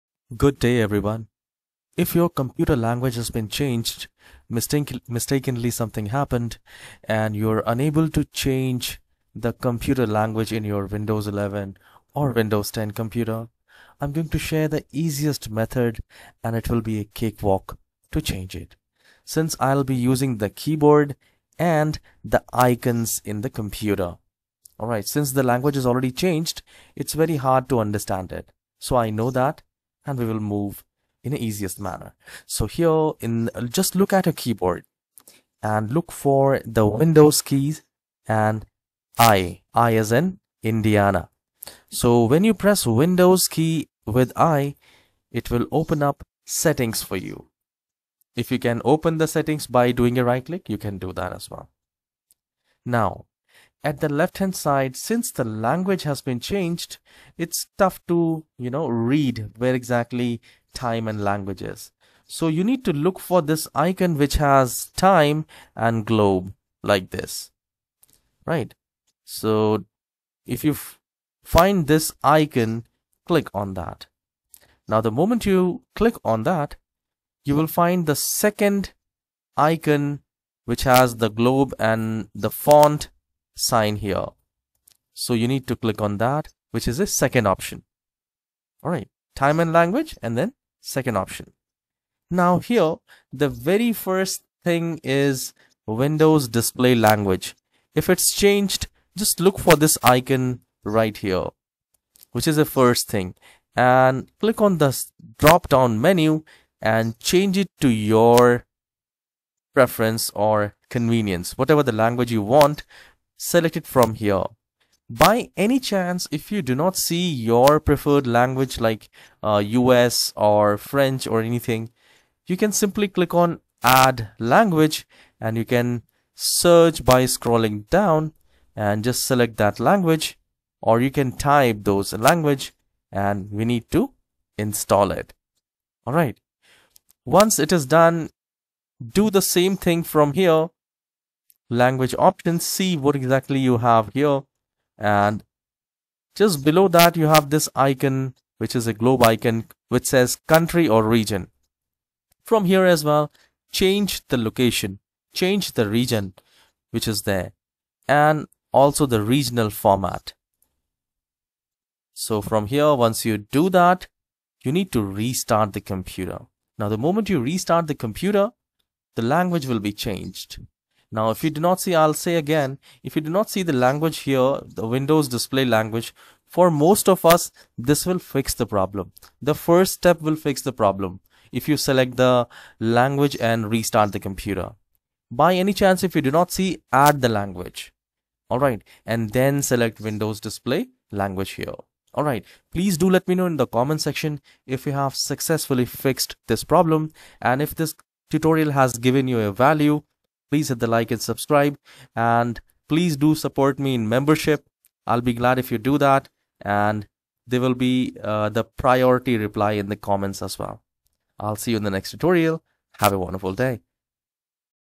How to Change Display Language in Windows 11 | Step-by-Step Guide
Welcome to our comprehensive tutorial on "How to Change Display Language in Windows 11"! If you're looking to switch the display language on your Windows 11 PC, you've come to the right place. In this step-by-step guide, we'll walk you through the entire process, making it easy to change your system language in just a few minutes.

🔹 Key Topics Covered:

🔹 Why Change Your Display Language in Windows 11?

Personalize your user experience
Improve accessibility
Enhance productivity for multilingual users
Use Windows 11 in your native language